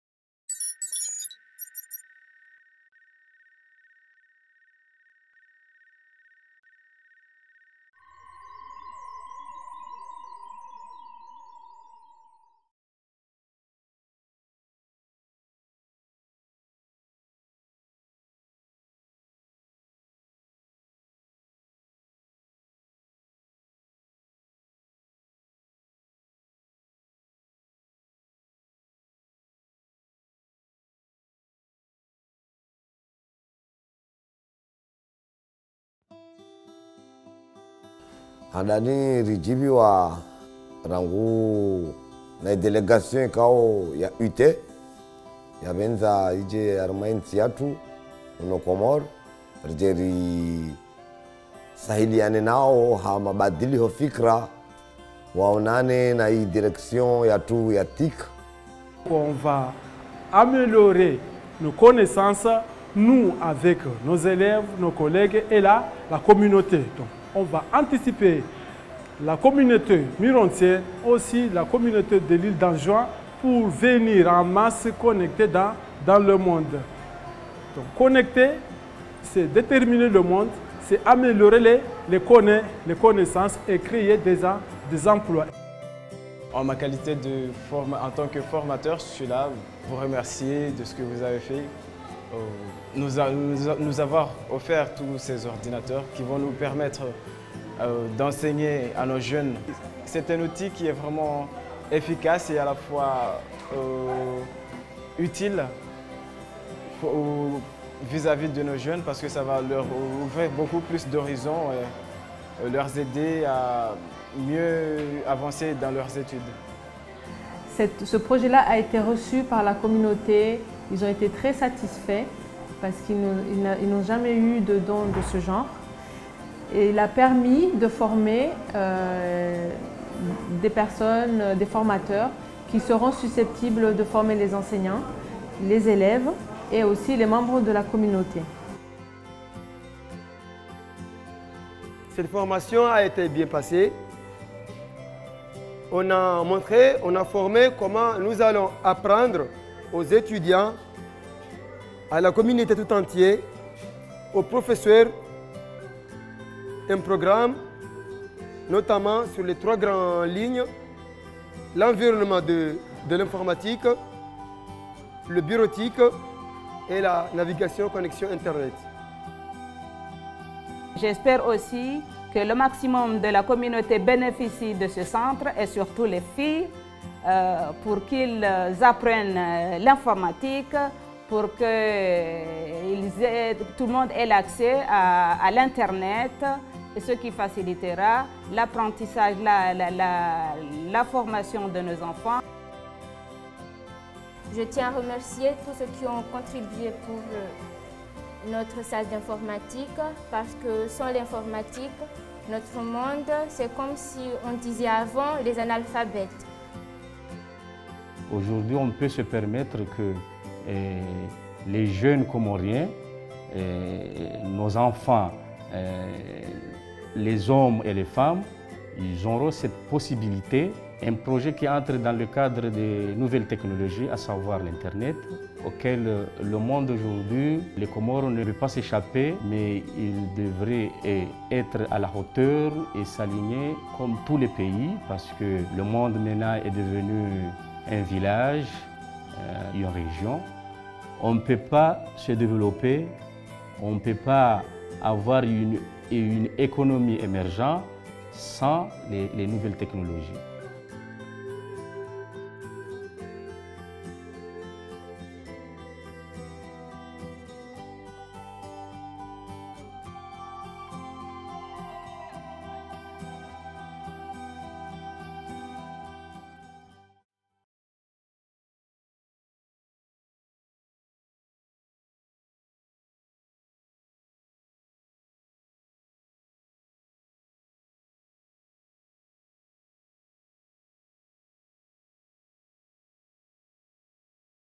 Comoros: Connect A School Connect a Community
The Connect a School Connect a Community project was implemented by ITU and the Comoros Autorité Nationale de Régulation des TIC (ANRTIC) to promote broadband connectivity in schools in remote and rural areas of the Comoros. The connected schools have improved access and use of ICTs by school children and also enabled local community access to the Internet.